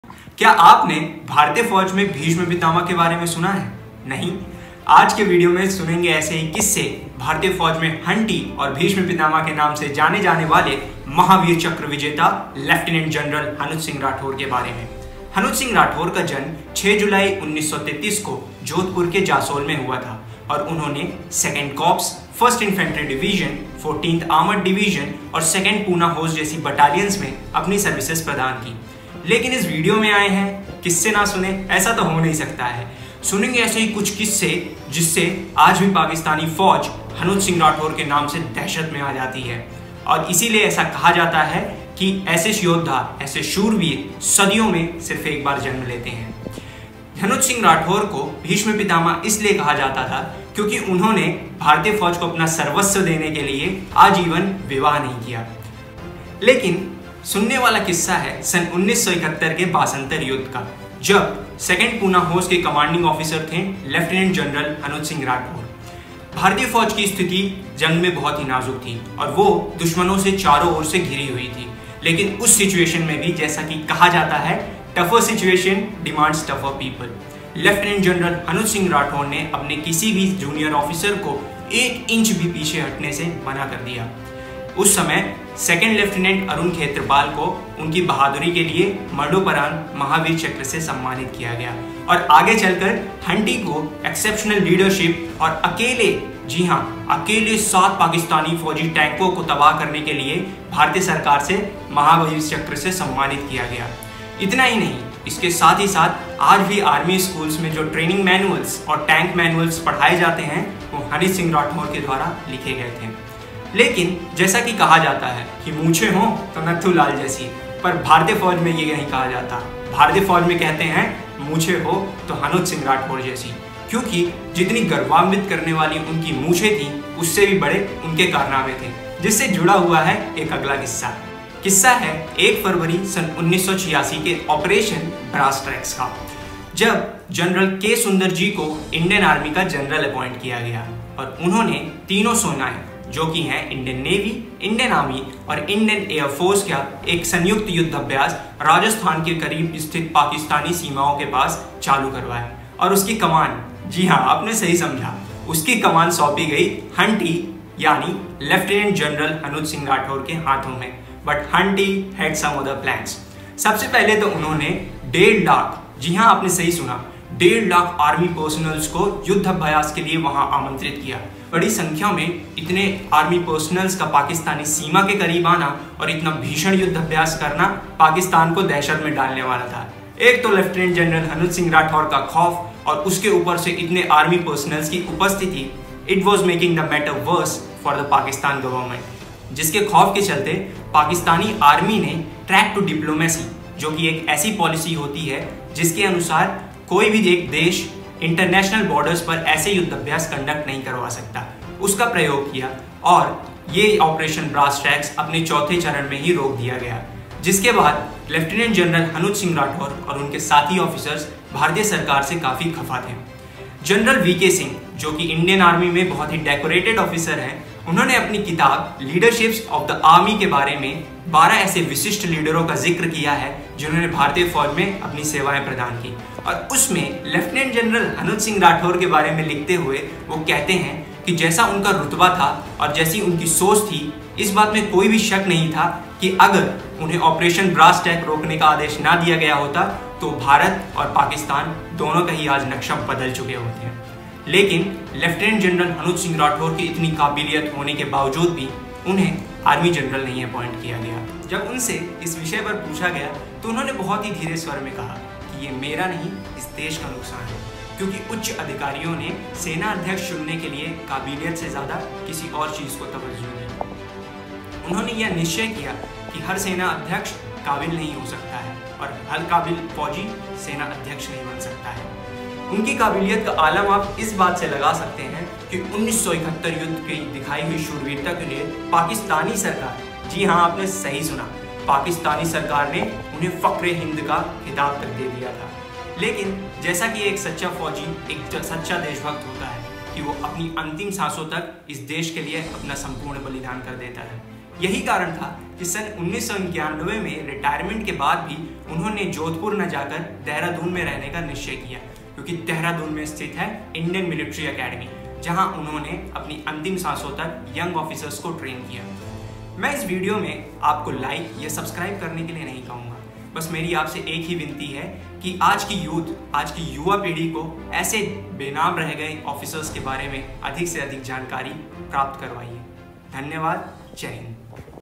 🔥❤️Story of India's "Huntie" aka "Lieutenant General Hanut Singh Rathore"!!❤️🔥
The courageous tales of Lieutenant "General Hanut Singh Rathore," a veteran of the Indian army, are told in the video up top. Sir, better known in the army as "Huntie," was one of the most effective leaders and bravest combatants in the "1971 Indo-Pak War" .In the Pakistani army ;during his time at war, Huntie was utterly feared. The "operation brass tracks" and "fight of basantar" are also discussed in the aforementioned video.